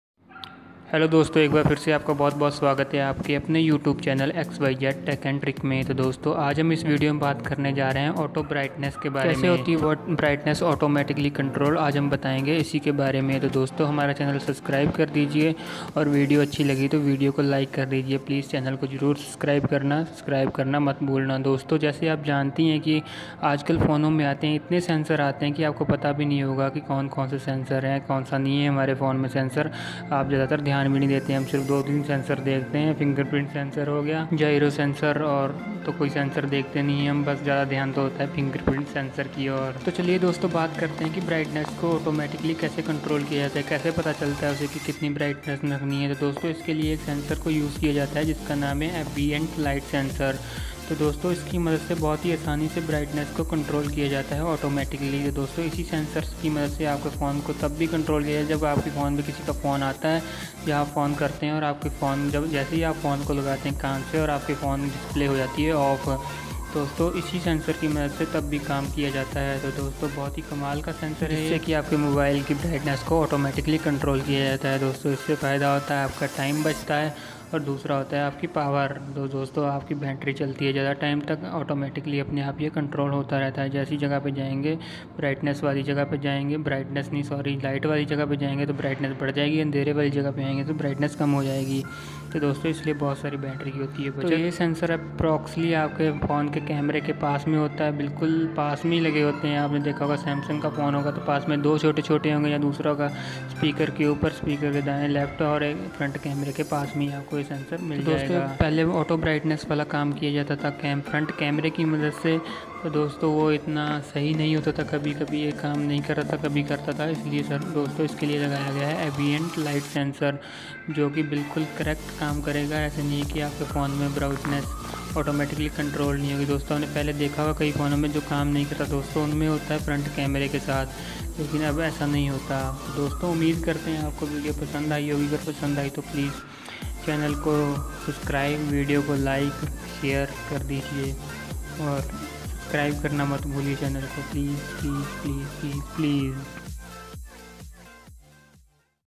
How automatic brightness sensor work in mobile , mobile sensor working, ambient light sensor,
Automatic brightnes kese hoti h control aaj kregai esi topic par baaat...